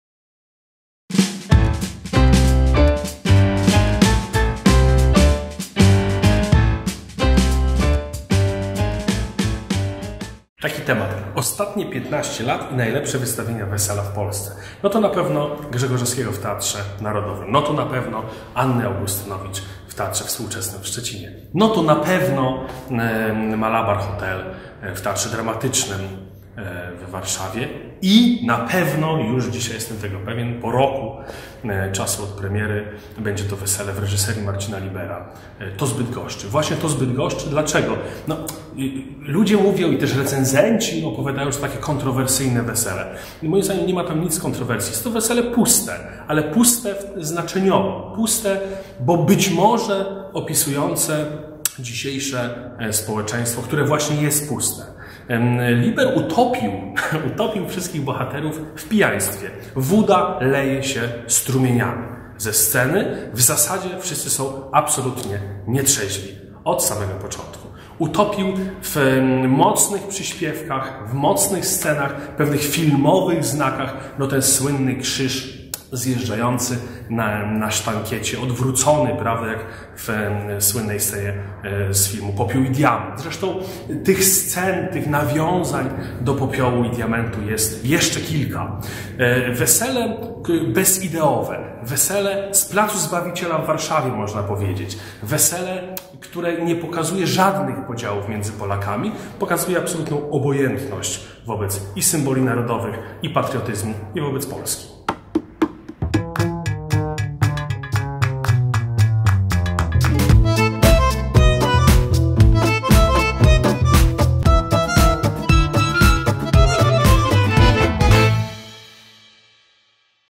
Wesele reż. Marcin Liber Teatr Polski w Bydgoszczy recenzuje Bartłomiej Miernik
Recenzja Bartka Miernika do spektaklu "Wesele" w Teatrze Polskim im. Hieronima Konieczki w Bydgoszczy.
Reżyseria:         Marcin Liber
scenografia: Mirek Kaczmarek
dramaturgia: Paweł Sztarbowski
muzyka: Filip Kaniecki
asystent reżysera: Maciej Pesta
Obsada:
Marysia, Widmo  – Karolina Adamczyk
Nos          – Michał Czachor
Rachela  – Joanna Drozda
Ojciec  – Mieczysław Franaszek
Wojtek  – Paweł L. Gilewski
Czepiec  – Mirosław Guzowski
Wernyhora  – Michał Jarmicki
Ksiądz  – Marian Jaskulski
Jasiek  – Artur Krajewski
Chochoł  – Magdalena Łaska
Poeta, Rycerz  – Mateusz Łasowski
Radczyni  – Alicja Mozga
Dziad, Upiór  – Roland Nowak
Pan Młody, Hetman  – Maciej Pesta
Haneczka, Isia  – Aleksandra Pisula
Żyd  – Jerzy Pożarowski
Dziennikarz, Stańczyk  – Piotr Stramowski
Klimina  – Małgorzata Trofimiuk
Gospodarz  – Jakub Ulewicz
Gospodyni  – Małgorzata Witkowska
Panna Młoda  – Julia Wyszyńska
Kasper, Kuba  – Marcin Zawodziński
Maryna  – Agnieszka Żulewska